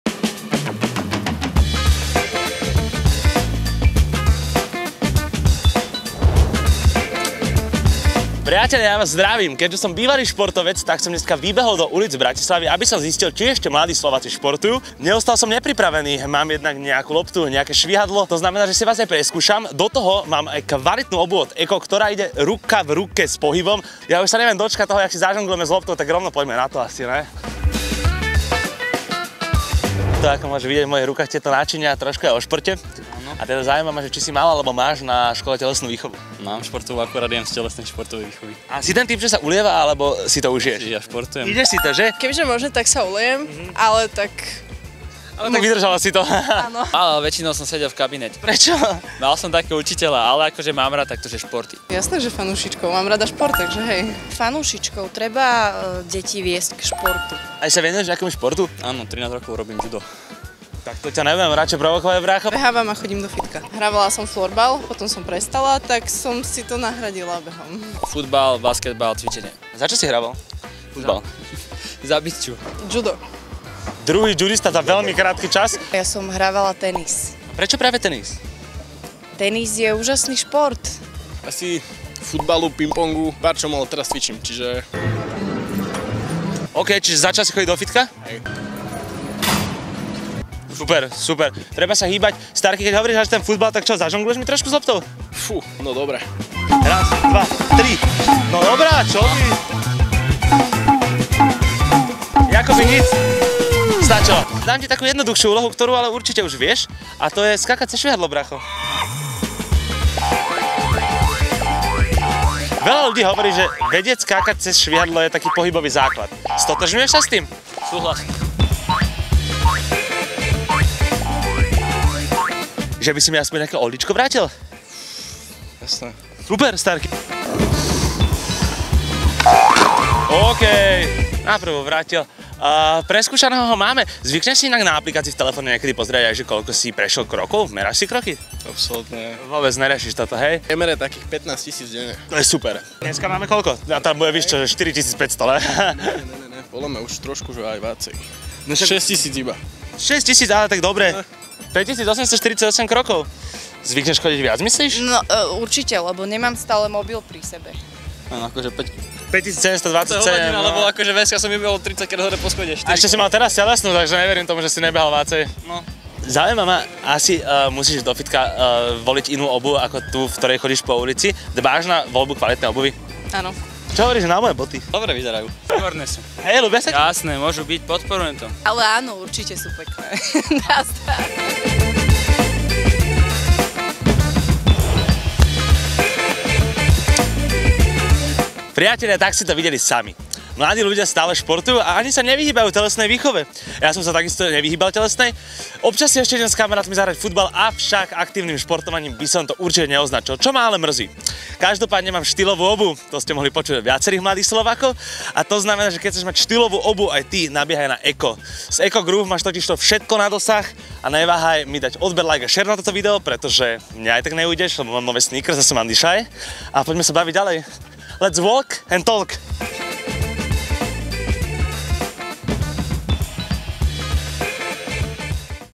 Zvládol by si tieto základné cviky z telesnej ? (TO DÁŠ 🤣)
Šajmo tentokrát zisťoval, ako na tom boli mladí ľudia v uliciach s telesnou výchovou, akým športom sa venujú a či si merajú napríklad aj počet denných krokov na smart zariadeniach. No a aby nezostalo len pri slovách, pripravil si pre nich aj športové aktivity. K tomu mu poslúžili aj vymakané tenisky od ECCO Gruuv, ktoré idú ruka v ruke s pohybom.

Moderoval: Šajmo
Natočil a postrihal: Pali
Produkcia: Kubo